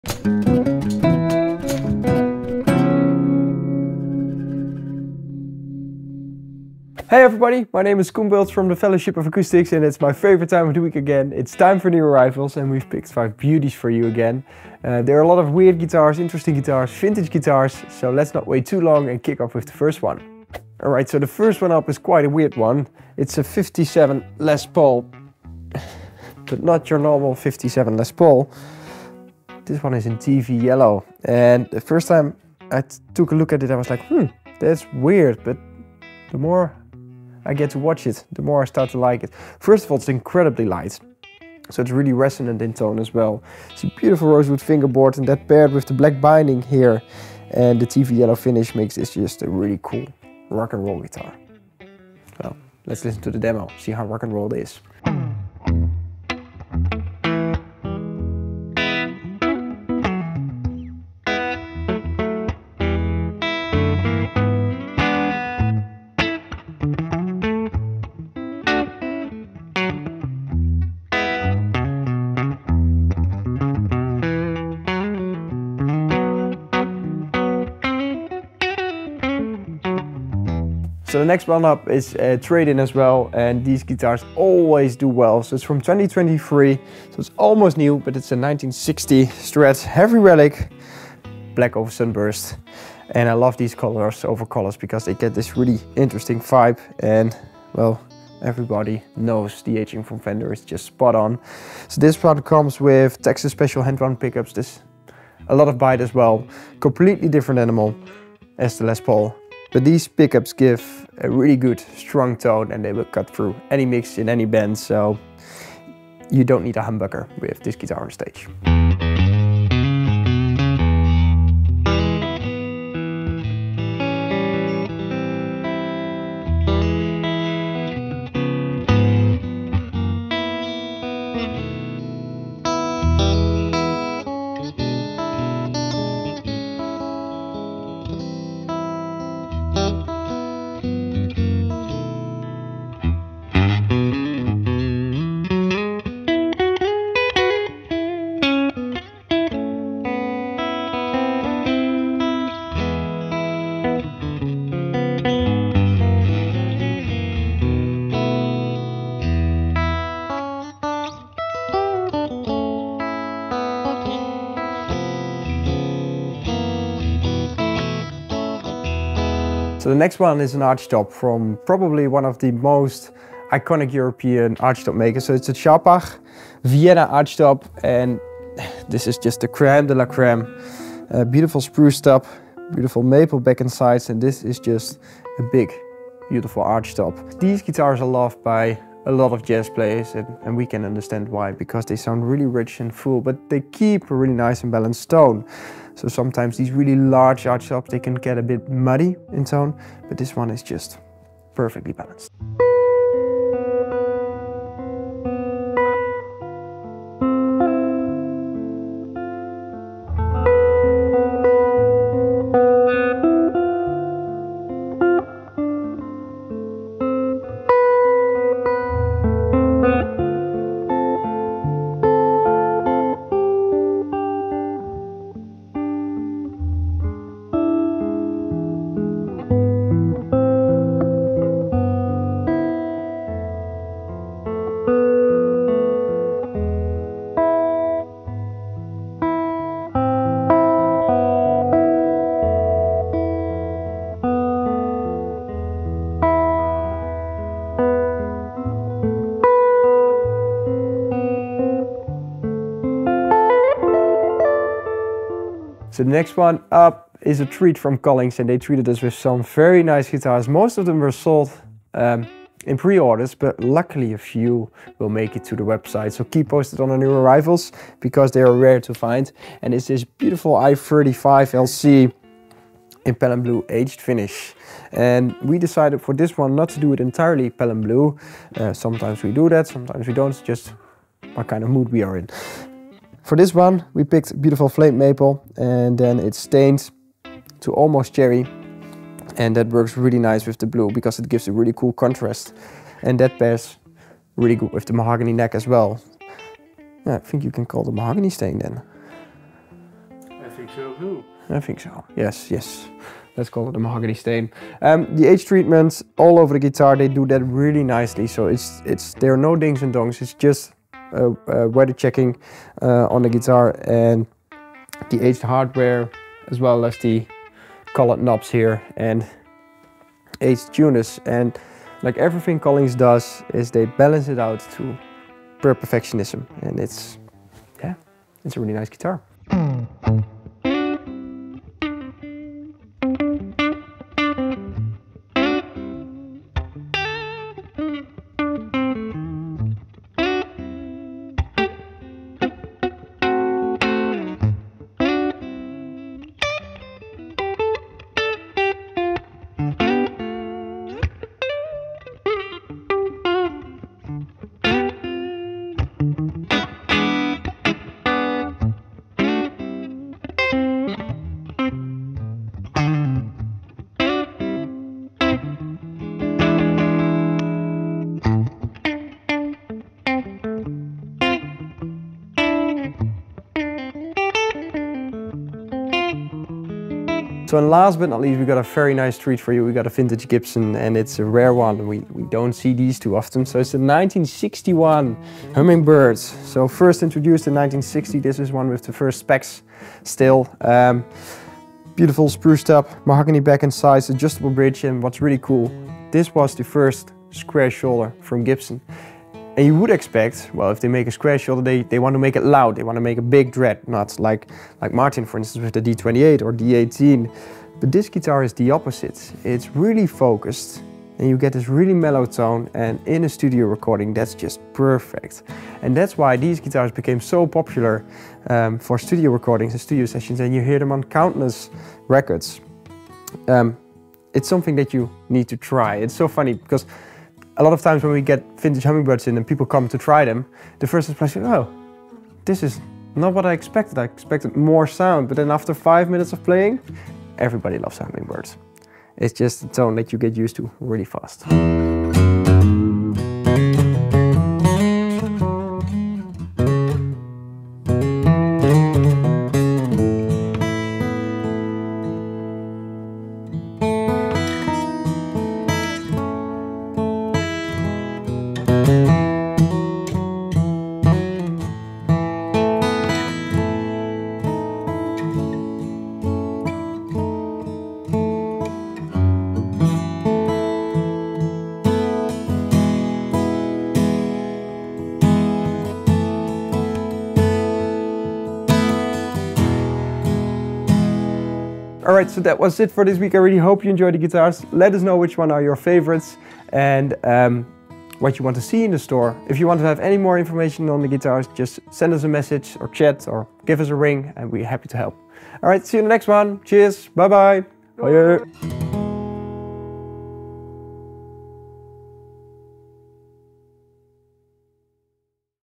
1961 Gibson Hummingbird & Collings I35LC Pelham Blue | New Arrivals #260
🎸 A Gibson Hummingbird from its introduction year, a TV Yellow Les Paul, a Sharpach archtop and more! Coen has got 5 really cool guitars to show you this week!

00:00 - Intro
00:20 - Gibson TV Yellow Specs
00:54 - Gibson TV Yellow Demo
01:20 - Fender Strat Specs
01:58 - Fender Strat Demo
02:49 - Theo Scharpach Specs
03:22 - The Scharpach Demos
04:15 - Collings I35LC Specs
05:29 - Collings I35LC Demo
06:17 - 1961 Specs Gibson Hummingbird
07:21 - Gibson Hummingbird Demo
08:01 - See ya!

"New Arrivals" every Friday @ 16:00! / New video every Tuesday @ 16:00!

More guitar goodness with less talking and more playing?